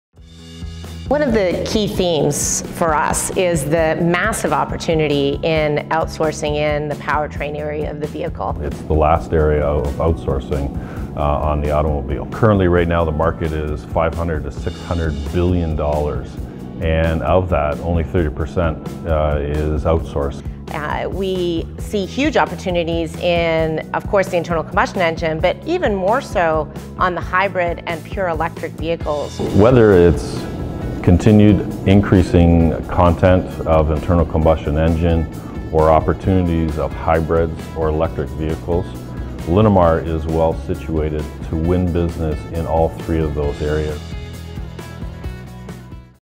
Key Investor Themes: Powertrain Outsourcing
Linda Hasenfratz, CEO, is joined by key members of Linamar’s executive and management group to discuss the Key Investment Highlights that make Linamar a compelling story for long-term value-oriented investors.